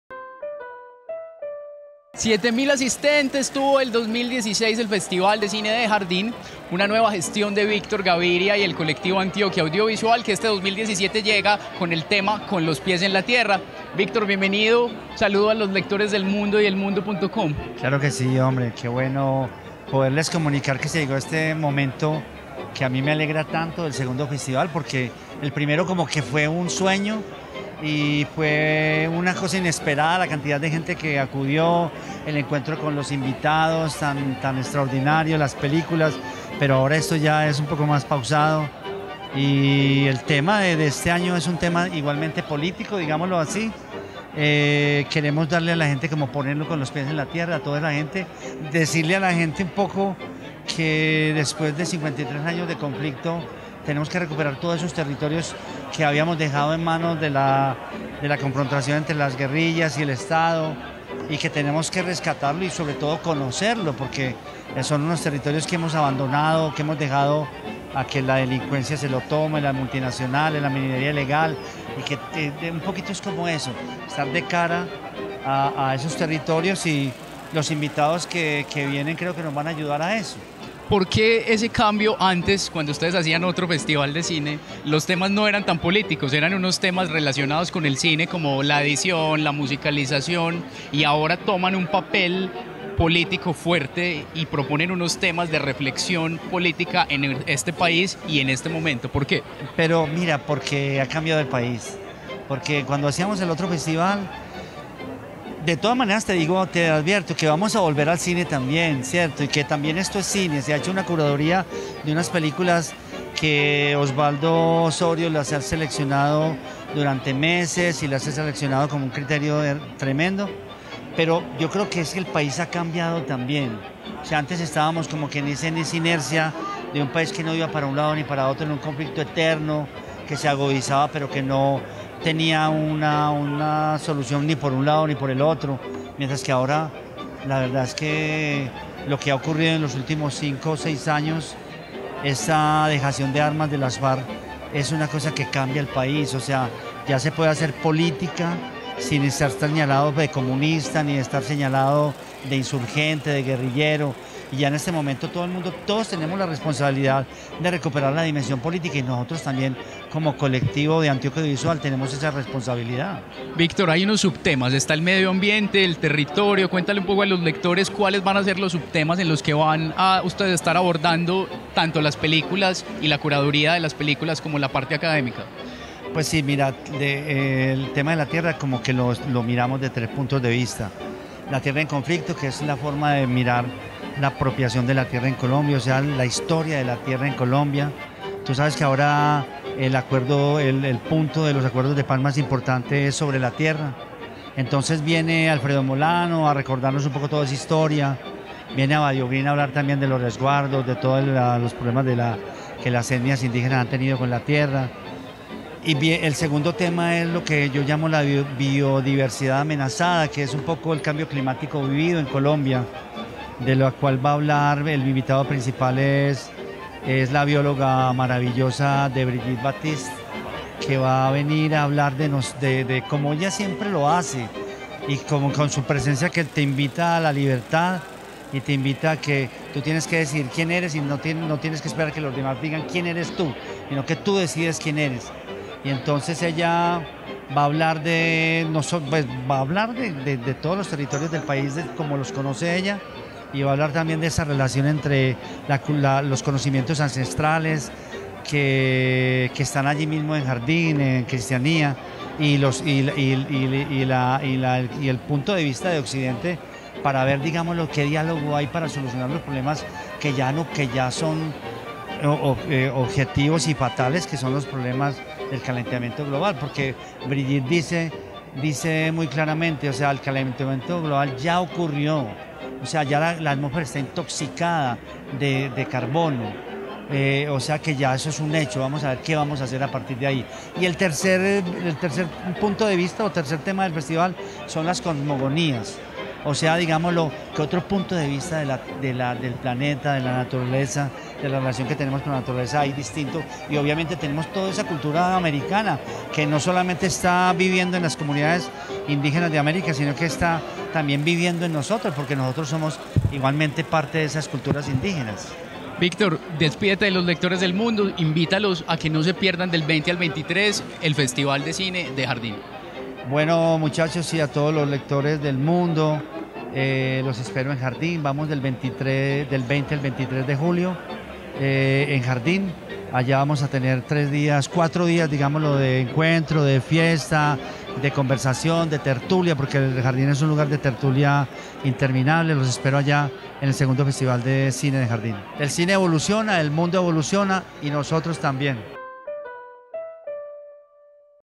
Así será el segundo Festival de Cine de Jardín
Víctor Gaviria, director del Festival de Cine de Jardín, le contó a EL MUNDO todos los detalles del segundo año del certamen, que en 2016 reunió a 7.000 personas. Este 2017, el Festival será realizado del 20 al 23 de julio y su tema central será: "Con los pies en la tierra", una reflexión sobre los territorios del país, luego del proceso de paz.
Contenidos Digitales Periódico El Mundo